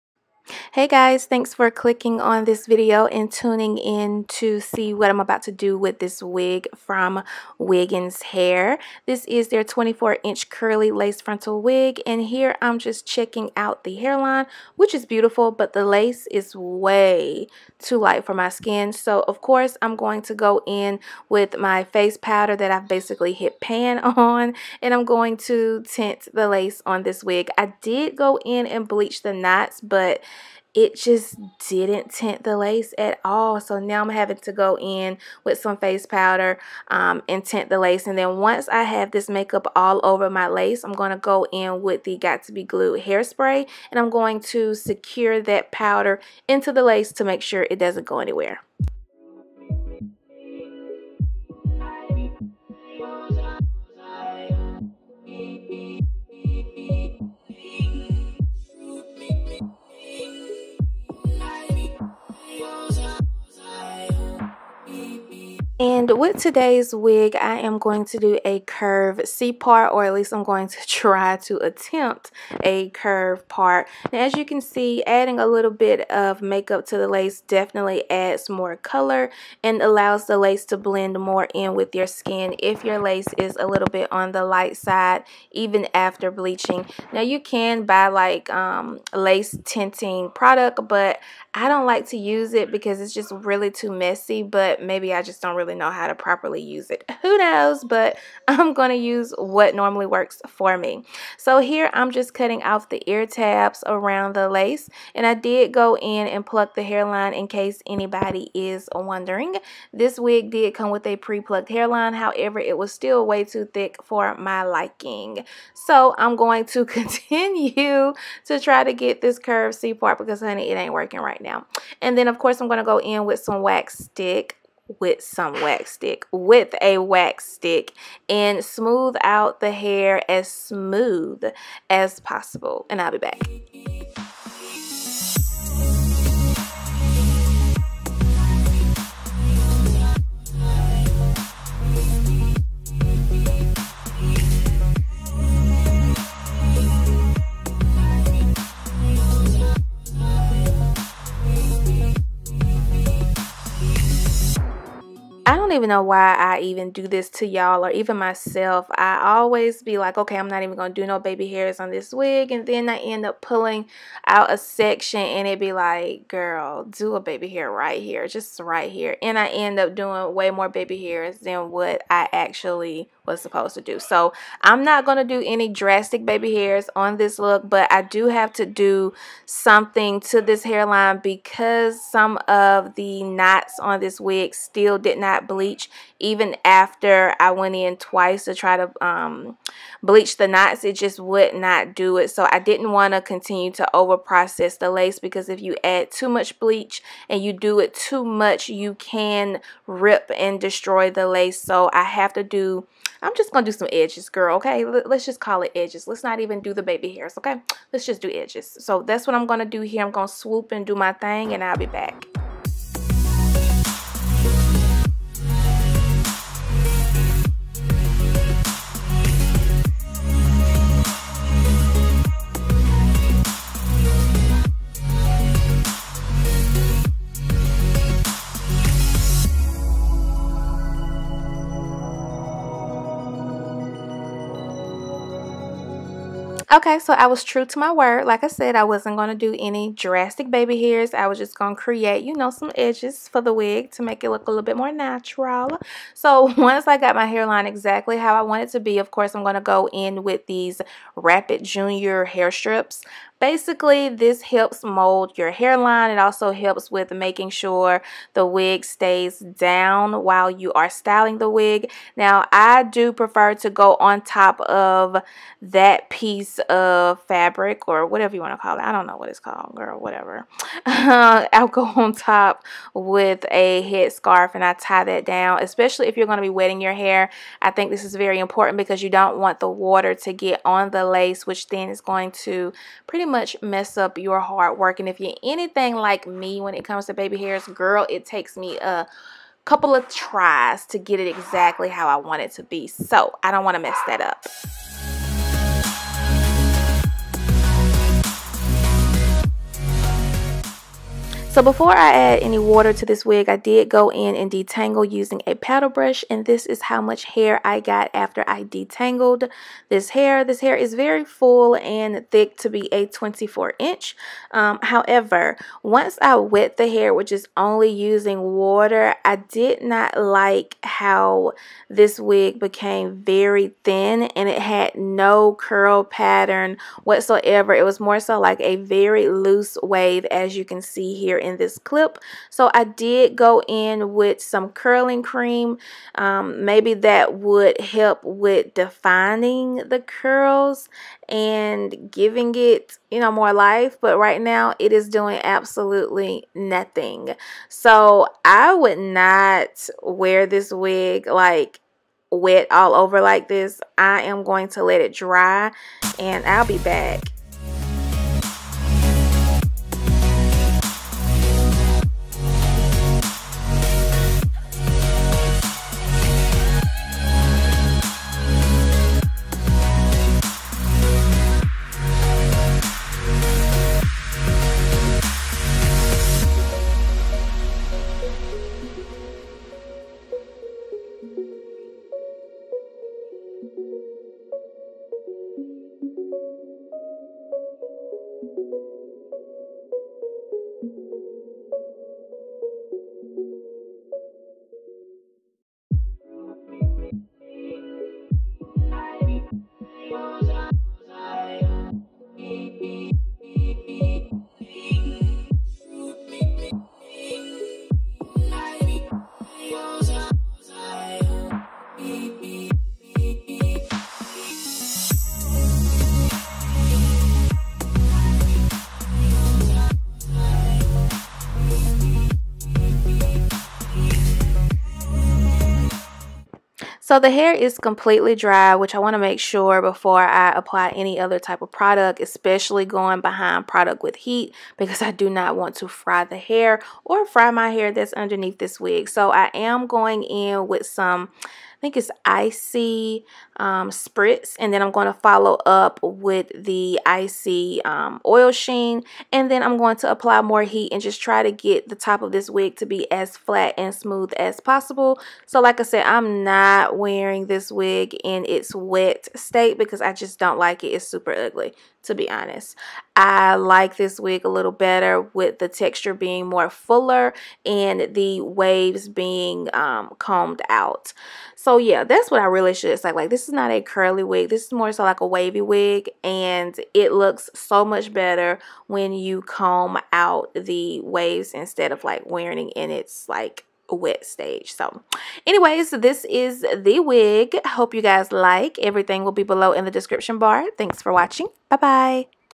Start to finish How I Melt And Blend Lace Frontal Wig Beginner friendly! Ft Wiggins Hair
დ WigginsHair დ
Hair details:loosedeepwave lace frontal wig 180% density 24 inches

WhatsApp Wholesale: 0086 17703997